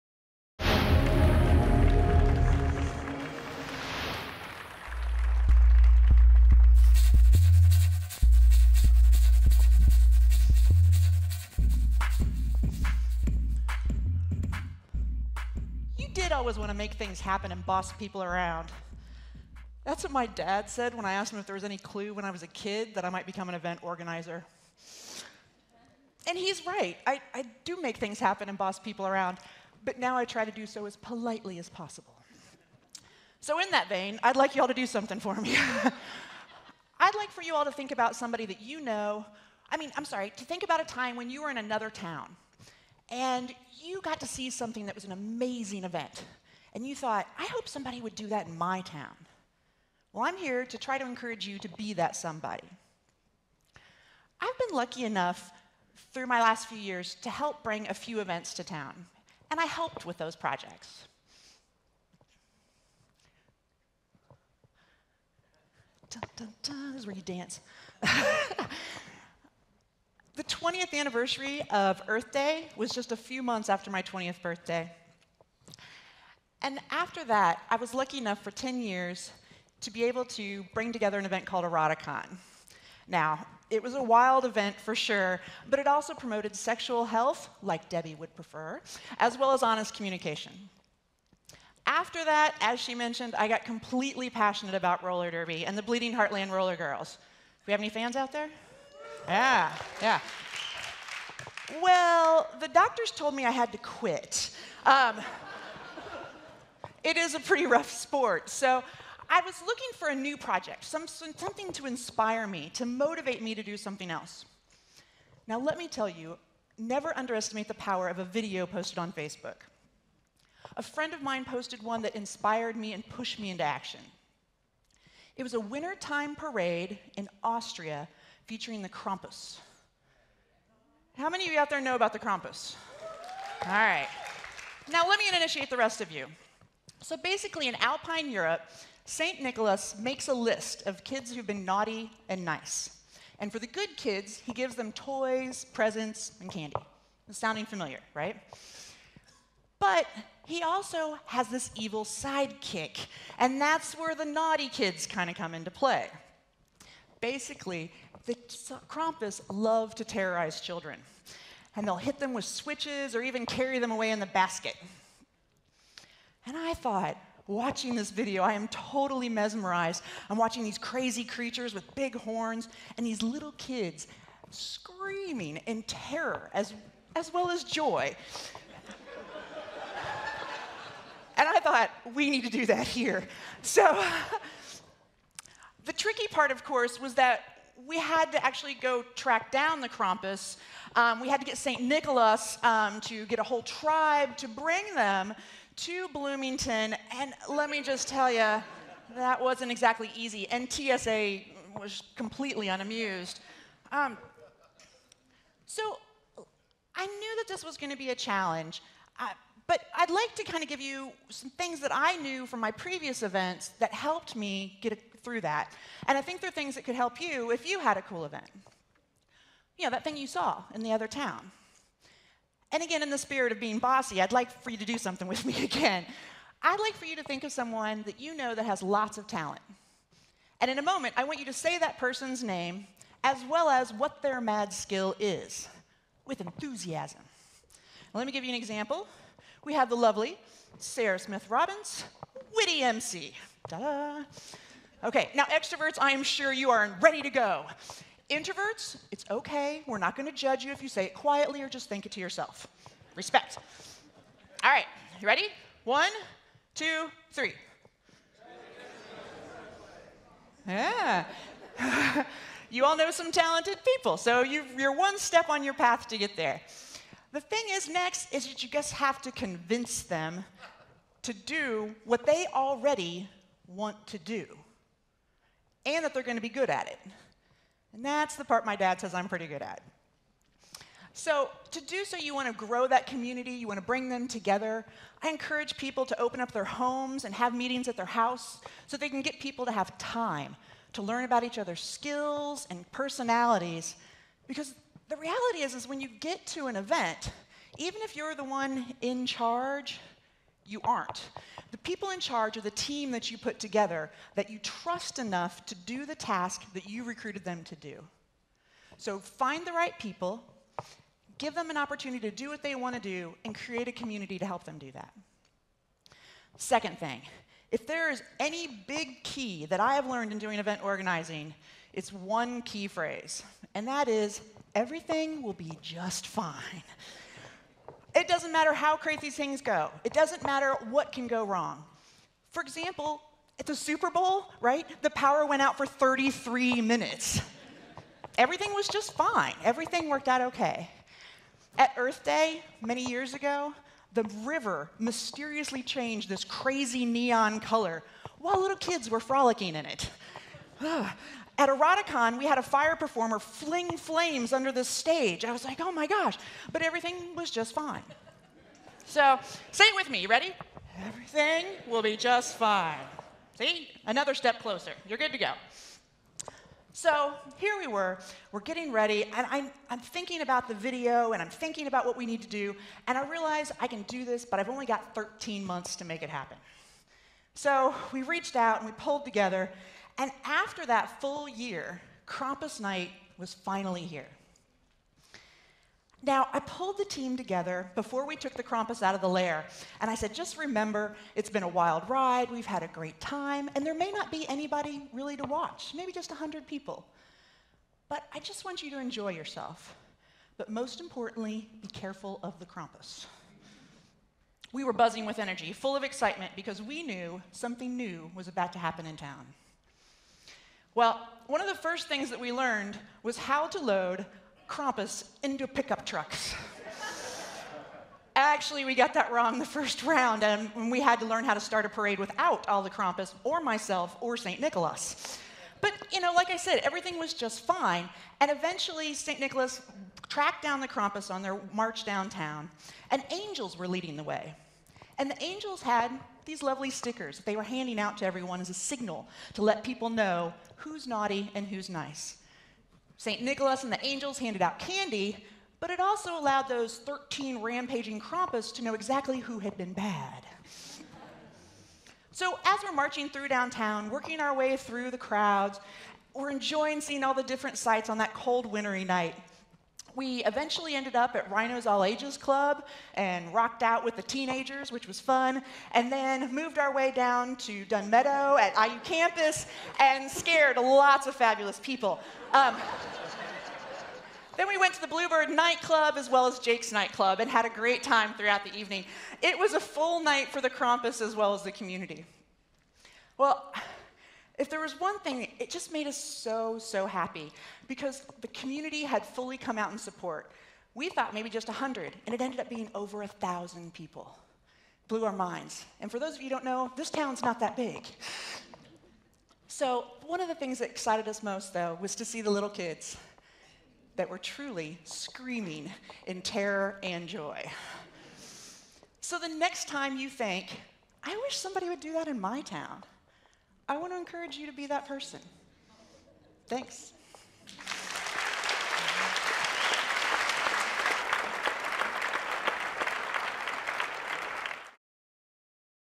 Want It? Make It Happen: Kelly McBride at TEDxBloomington
In 2012, Kelly E. McBride founded KLAW, the Krampus Legend and Arts Workshop, which brought the mysterious Krampus to Bloomington. Last December in their inaugural appearance, the Krampus were enjoyed by over 1,000 people. Krampus Night 2013 will be even more enchanting. An avid quad roller skater, Kelly co-founded Bloomington's Bleeding Heartland Rollergirls in 2006, one of the first 50 roller derby leagues in the country and the first outside a major metropolitan area. She also co-starred in "Eight Wheels of Death," the world's first roller derby zombie romantic comedy. In 1995 she founded the group Lascivious Exhibitions and created EROTICON, an event to promote sexual health and open, honest communication. For more than a decade, LE awarded over $10,000 in grants, covered almost 40 people in chocolate, distributed over 35,000 free condoms, and educated multitudes of lovers. McBride has resided in Bloomington for 25 years.